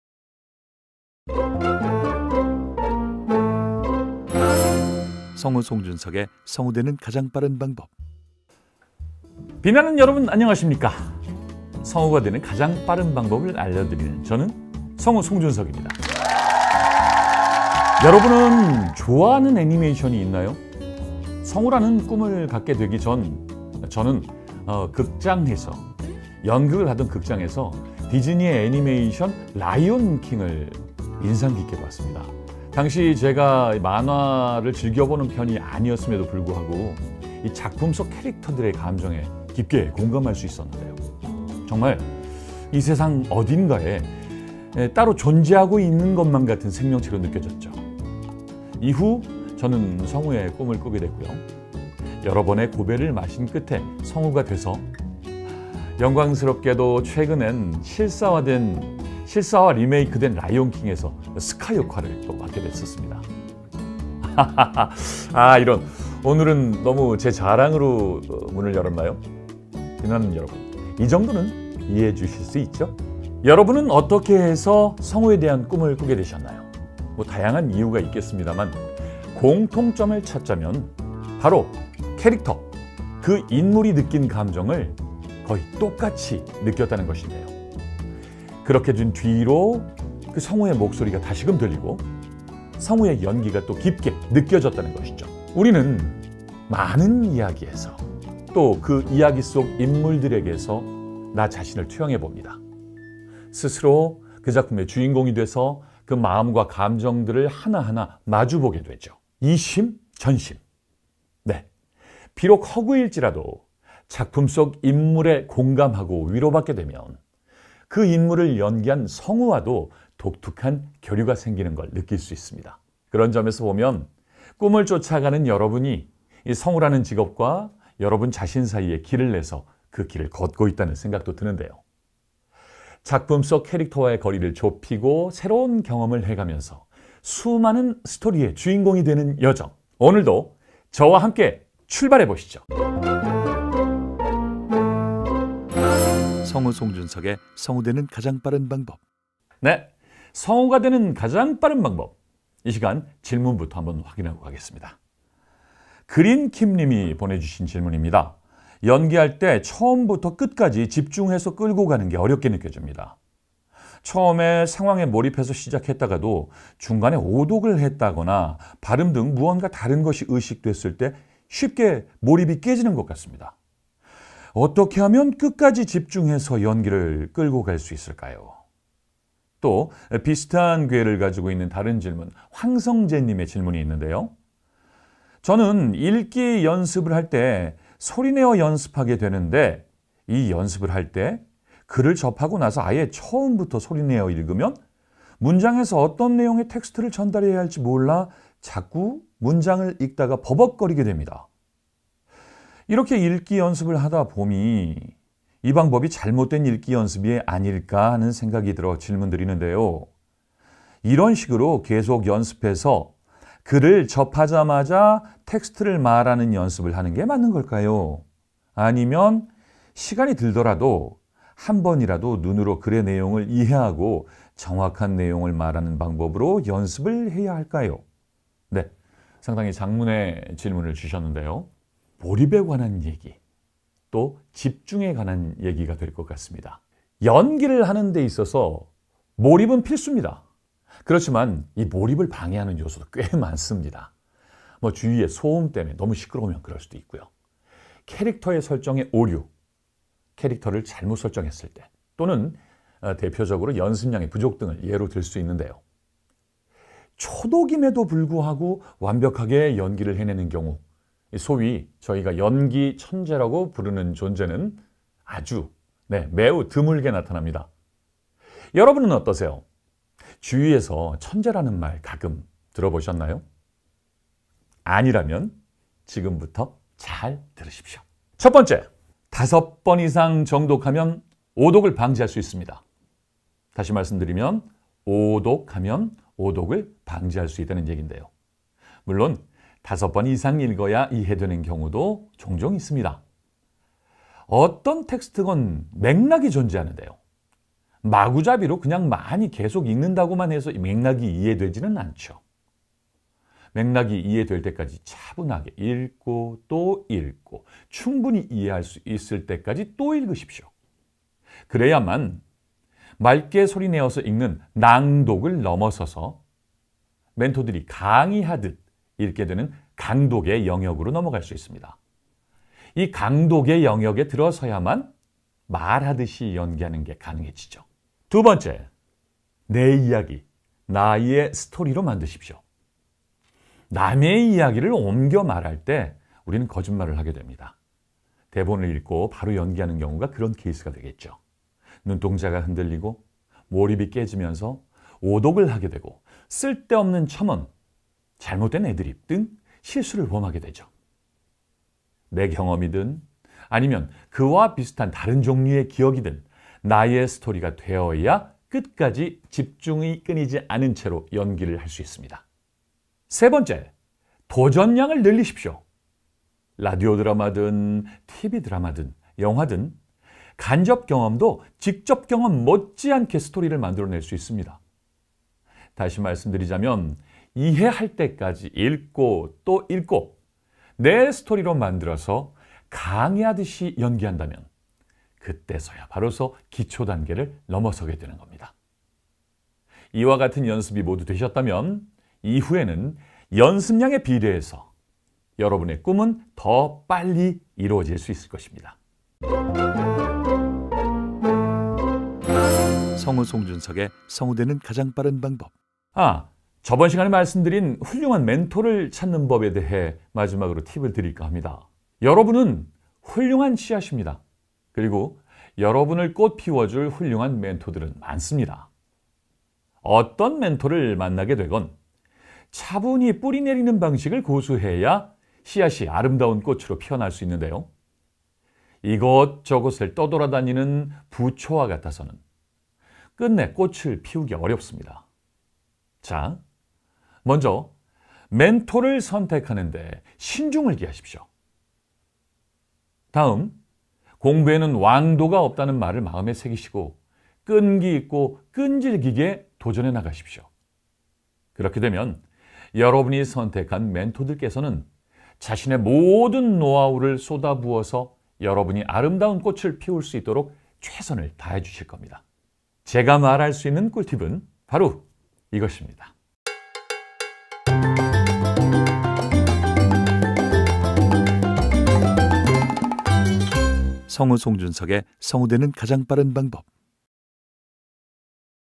'몰입과 집중' 성우수업21 - 13교시 - 성빠방(성우 되는 가장 빠른 방법)
성우수업21-13교시  - 성우 되는 가장 빠른 방법(성빠방)
- 성우수업 주제 ; 연기의 몰입과 집중

성우공부의 시작! [@성우특별기초반]